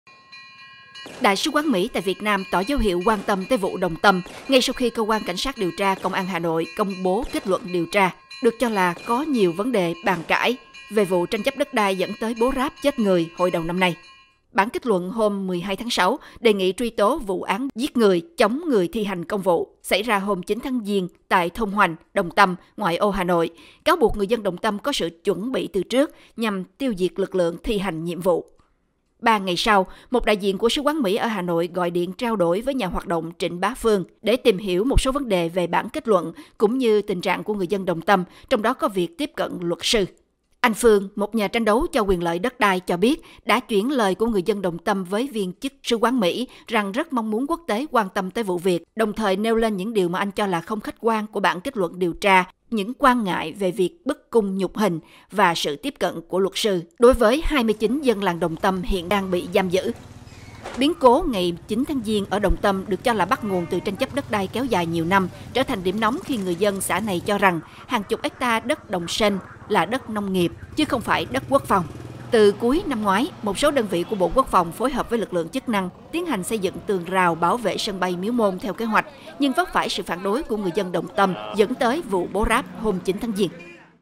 ĐSQ Mỹ quan tâm vụ Đồng Tâm sau khi công an ra kết luận điều tra (VOA)
Tin tức Nếu không vào được VOA, xin các bạn hãy vào để vượt tường lửa. Đại sứ quán Mỹ tại Việt Nam tỏ dấu hiệu quan tâm tới vụ Đồng Tâm ngay sau khi cơ quan cảnh sát điều tra Công an Hà Nội công bố kết luận điều tra, được cho là “có nhiều vấn đề bàn cãi”, về vụ tranh chấp đất đai dẫn đến bố ráp gây chết người hồi đầu năm nay.

Cơ quan điều tra Công an Hà Nội công bố bản kết luận điều tra vụ án hình sự hôm 12/6, trong đó đề nghị truy tố vụ án giết người, chống người thi hành công vụ xảy ra hôm 9/1 tại thôn Hoành, Đồng Tâm ở ngoại ô Hà Nội. Bản kết luận điều tra dài 47 trang cáo buộc người dân Đồng Tâm “có sự chuẩn bị từ trước” qua việc “tập hợp lực lượng, đóng góp tiền mua vũ khí” cũng như lên kế hoạch “phân công nhiệm vụ từng đối tượng” và “chủ động dùng vũ khí tấn công quyết liệt” nhằm “tiêu diệt lực lượng thi hành nhiệm vụ.”

Ba ngày sau đó, một đại diện của sứ quán Mỹ ở Hà Nội đã gọi điện trao đổi với nhà hoạt động Trịnh Bá Phương để tìm hiểu một số vấn đề về bản kết luận điều tra cũng như tình trạng của người dân Đồng Tâm, trong đó có việc tiếp cận luật sư và tâm lý của họ.

“Viên chức Đại sứ quán có nói họ đã đọc hết bản kết luận điều tra đó,” anh Phương, một người tranh đấu quyền đất đai của làng Dương Nội, cho biết. “Họ cũng nói đã đọc bài viết của tôi đăng trên Facebook cá nhân phân tích các điểm phi lý trong bản kết luận điều tra, trong đó trọng tâm là về sự phi lý trong cự ly bắn cụ Kình và việc can xăng chỉ còn có 5-6 lít mà lại có thể thiêu cháy than hoá được 3 viên cảnh sát cơ động. Đó là điều phi lý.”

Trong vụ bố ráp rạng sáng ngày 9/1 tại Đồng Tâm, cụ Lê Đình Kình, một đảng viên và là thủ lĩnh tinh thần của người dân làng trong cuộc đấu tranh “bảo vệ đất” bị lực lượng an ninh của chính quyền bắn thiệt mạng. Công an Hà Nội kết luận rằng người đàn ông 84 tuổi “đã sử dụng tuýp sắt gắn dao phóng lợn, 1 quả lựu đạn tấn công lược lượng làm nhiệm vụ” và do đó hành vi của ông Kình cấu thành tội “giết người.”.

Vẫn theo bản kết luận điều tra đưa ra hôm 12/6, người dân Đồng Tâm đã “nhiều lần đổ xăng xuống hố và châm lửa đốt, là nguyên nhân trực tiếp làm 3 chiến sỹ công an hy sinh.”

Trao đổi với VOA sau khi nhận được bản kết luận điều tra của cơ quan công an, các luật sư nhận tham gia bào chữa trong vụ án ở Đồng Tâm nói rằng họ hơi bất ngờ vì “kết luận điều tra được đưa ra quá nhanh”. Theo LS Ngô Anh Tuấn, thông thường phải mất khoảng một năm để đưa ra bản kết luận đối với một vụ án có tới 29 người bị truy tố như ở Đồng Tâm.

Anh Phương cho biết anh cũng trao đổi với tuỳ viên sứ quán Mỹ, mà anh không muốn đưa ra danh tính, về việc tiếp cận luật sư đối với những người Đồng Tâm bị truy tố.

LS Đặng Đình Mạnh, một người tham gia bào chữa vụ án này, cho VOA biết hôm 12/6 rằng ông và các luật sư khác đã được tiếp xúc với các bị cáo nhưng không được tiếp xúc trực tiếp và trong điều kiện “rất hạn chế”.

Trong cuộc nói chuyện với đại diện sứ quán Mỹ hôm 15/6, anh Phương cho rằng kết luận của cơ qua điều tra là không khách quan “vì cơ quan công an TP Hà Nội là cơ quan trực tiếp nổ súng bắn chết cụ Kình tại phòng ngủ mà họ lại nắm quyền điều tra truy tố như vậy cho thấy rằng chính họ đã vi phạm luật pháp Việt Nam, luật tố tụng hình sự. Anh cũng nêu quan ngại về việc bức cung nhục hình, một tình trạng mà theo anh rất phổ biến ở Việt Nam.

Biến cố hôm 9/1 ở Đồng Tâm được cho là bắt nguồn từ tranh chấp đất đai kéo dài nhiều năm và trở thành điểm nóng khi người dân xã này cho rằng hàng chục hecta đất đồng Sênh là đất nông nghiệp của xã chứ không phải đất quốc phòng. Từ ngày 31/12/2019, một số đơn vị của Bộ Quốc phòng phối hợp với lực lượng chức năng tiến hành xây dựng tường rào bảo vệ sân bay Miếu Môn theo kế hoạch nhưng vấp phải sự phản đối của người dân Đồng Tâm, dẫn đến vụ bố ráp hôm 9/1.

Hồi tháng 2 năm nay, Đại sứ Mỹ tại Hà Nội Daniel Kritenbrink nói trong một buổi gặp gỡ cộng đồng người Việt ở San Jose, California, rằng Hoa Kỳ không chỉ đứng ngoài quan sát biến cố Đồng Tâm mà có nhiều quan tâm đến vụ việc. Tuy nhiên, ông nói thật khó biết được sự thật về những gì xảy ra tại đó và lấy làm tiếc là có những người chết trong sự kiện và mong hai bên sẽ giải quyết vấn đề một cách ôn hoà và trong tinh thần pháp trị.

Anh Phương, người từng gặp đại diện sứ quán Mỹ ở Hà Nội hồi tháng 2 cũng để trao đổi về vụ Đồng Tâm, cho biết đã “chuyển lời của người dân Đồng Tâm với viên chức sứ quán Mỹ rằng rất mong muốn quốc tế quan tâm đến vụ việc này, bởi ở đất nước chúng tôi người dân rất khó có được sự công bằng với nền tư pháp như hiện nay.”